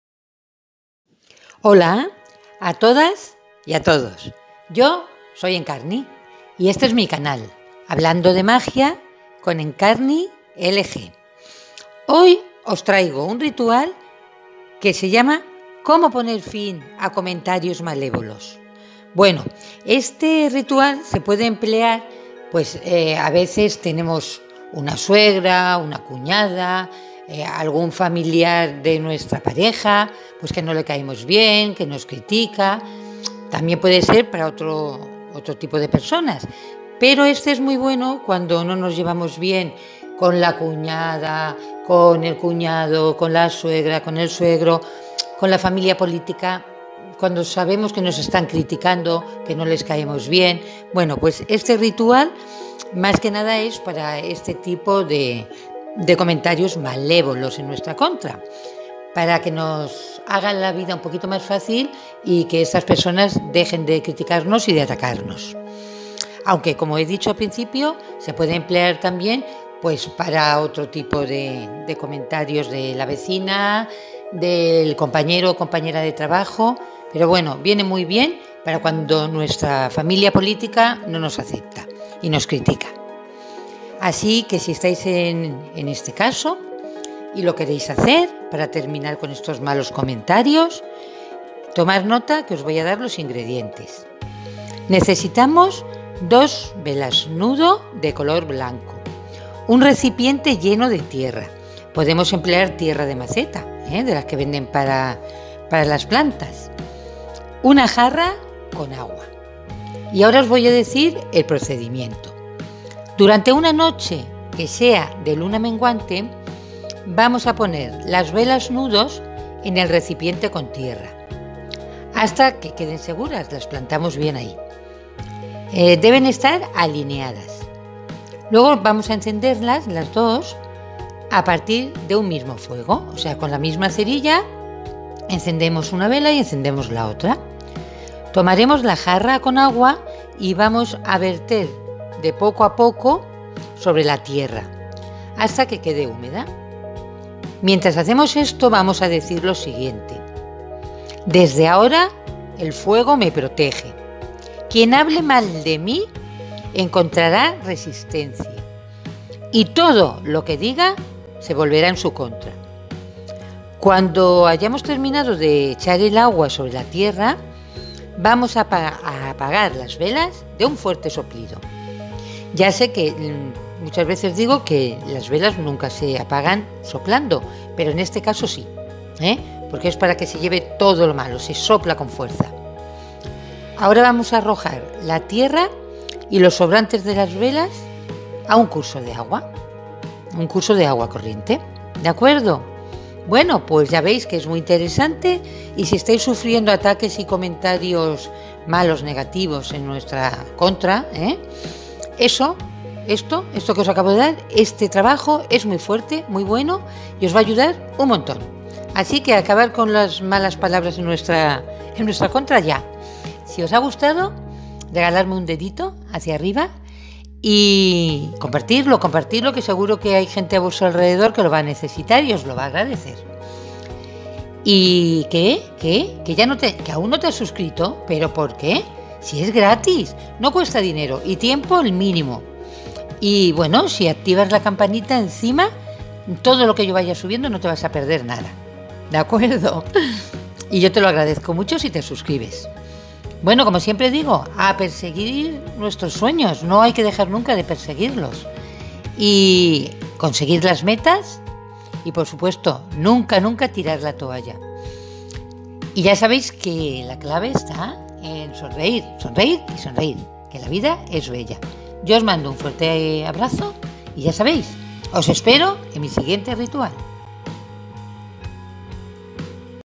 COMO PONER FIN A COMENTARIOS MALÉVOLOS
Terminar con los comentarios malévolos, las críticas ácidas provenientes de la familia, amigos, vecinos o compañeros de trabajo.